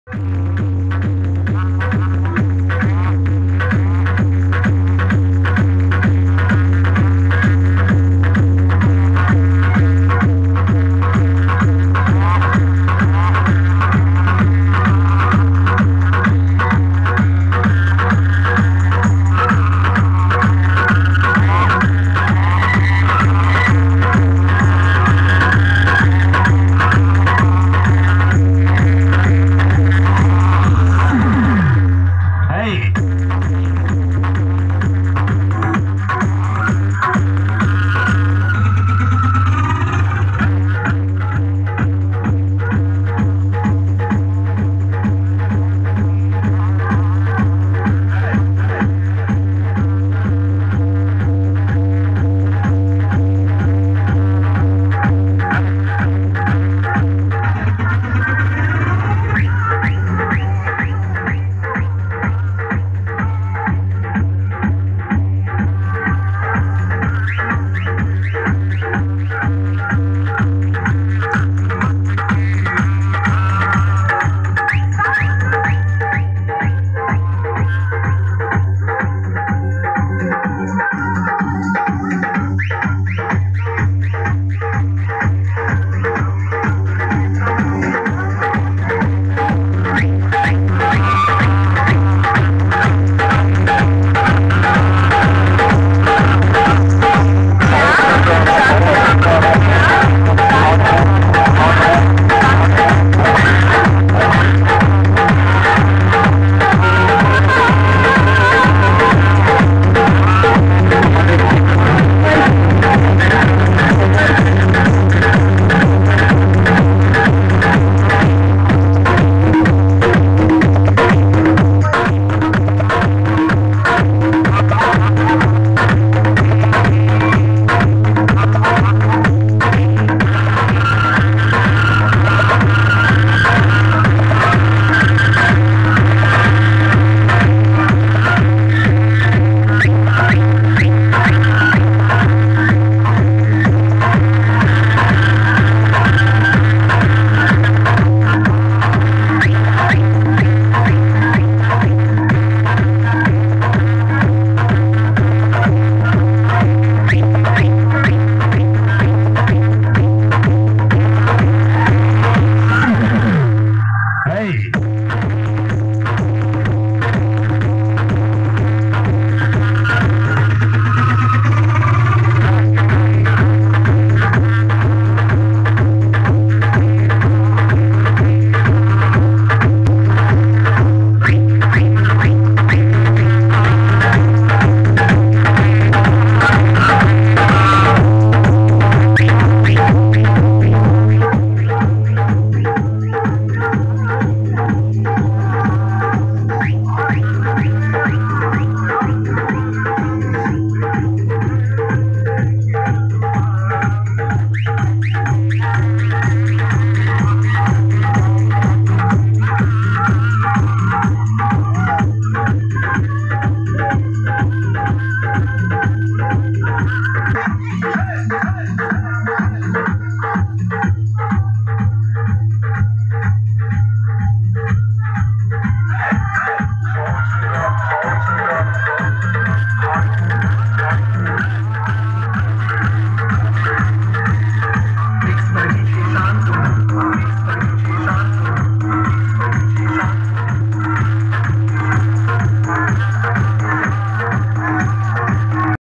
মোজাম সাউন্ড অপারেটর সাগর মূর্মু ও শুশান্তদা সব কিছু বাজিয়ে চেক করছে নিচ্ছিল পজিশানে বেড়োবার আগে
Mojam Sound
Sound Center Mojam
Mojam Sound Check Sound
Appearing Sagor Murmu
Appearing Susanto Da
Amit
Amit Roy

▶ Hag Tages : ◀